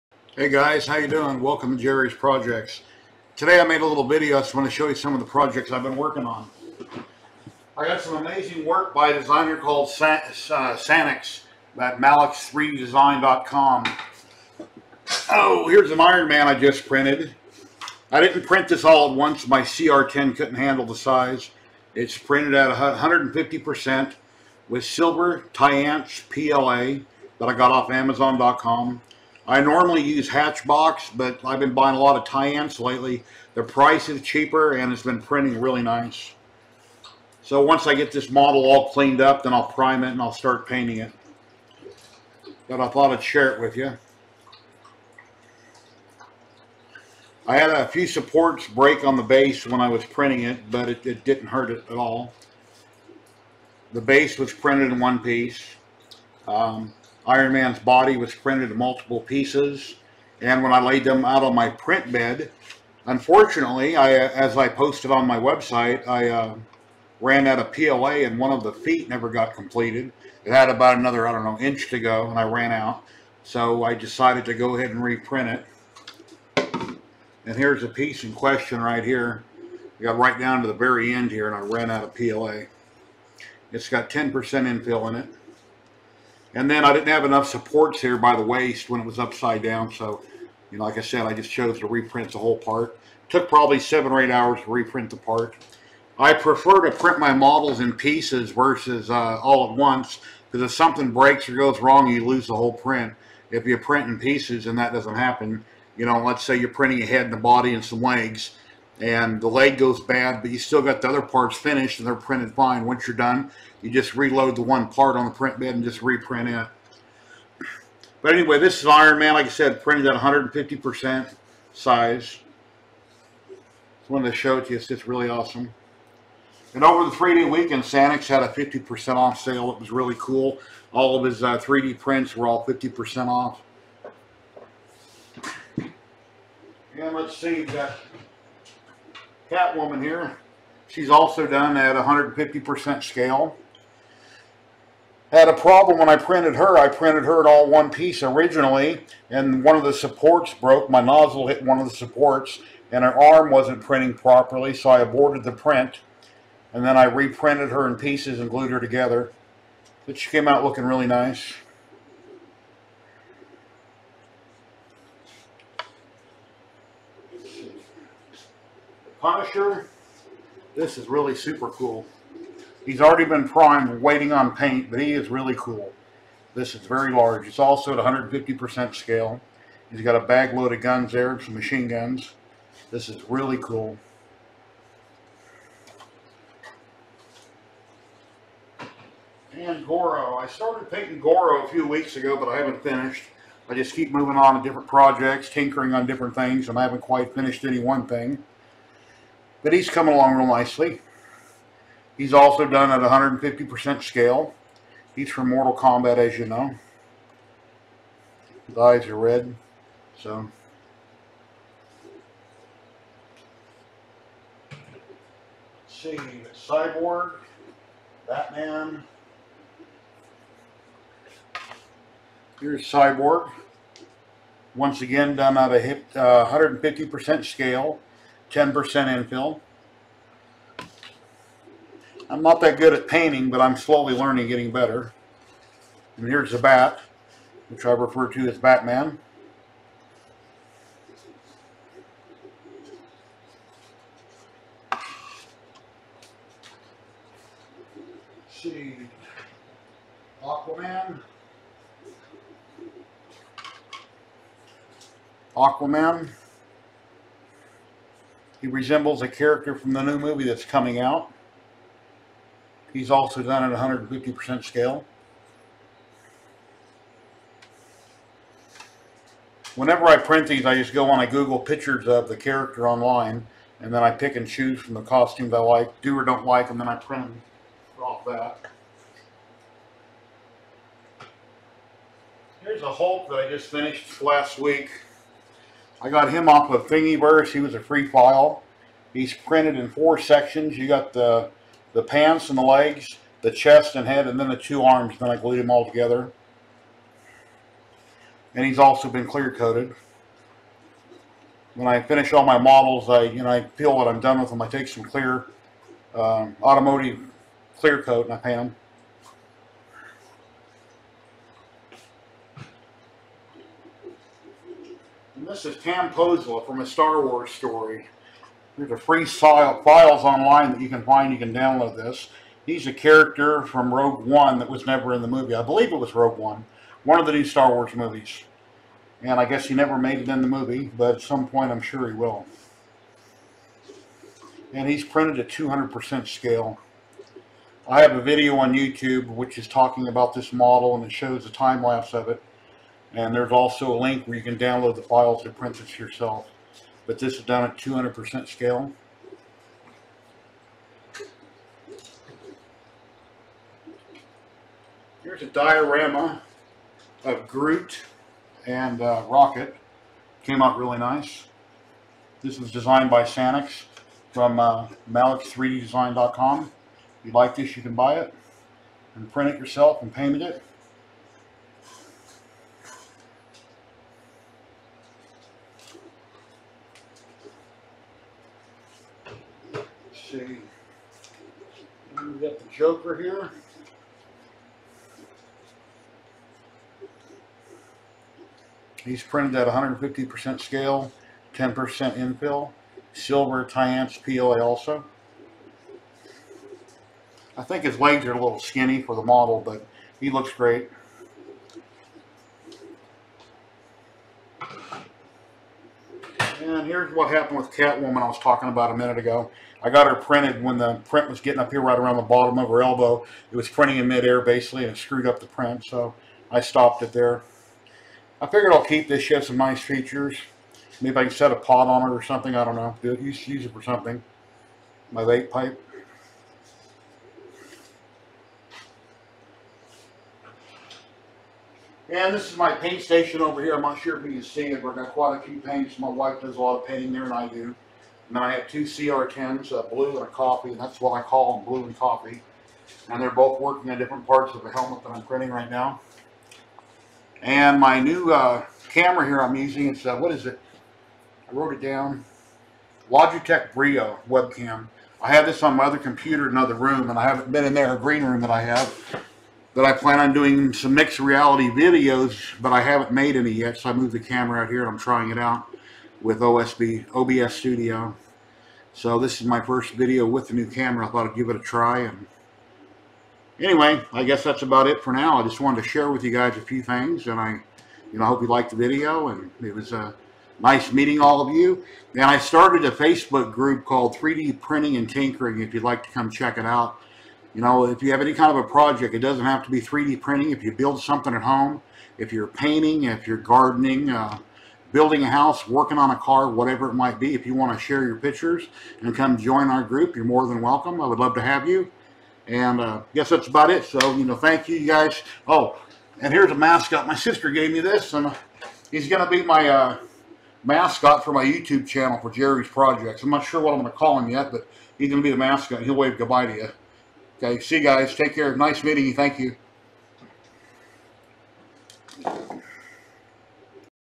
3D Printings on my CR10 Featuring Sanix Prints
3D Prints on my Creality CR 10.and featuring prints from SANIX

Amazon USA

Amazon CA

Amazon UK

Amazon AU
amzn.to/2mdaRvb

Created with Movie Studio Platinum